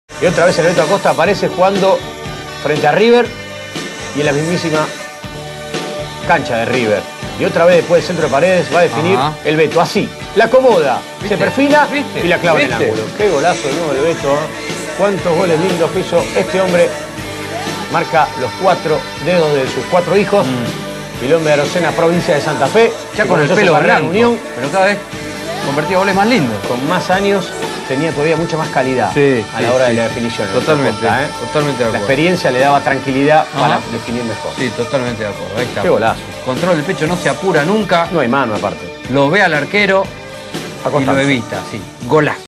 Alberto Beto Acosta - San Lorenzo vs River Plate - Apertura 2003
Gol de Alberto Beto Acosta jugando para  San Lorenzo partido vs River Plate en el año 2003.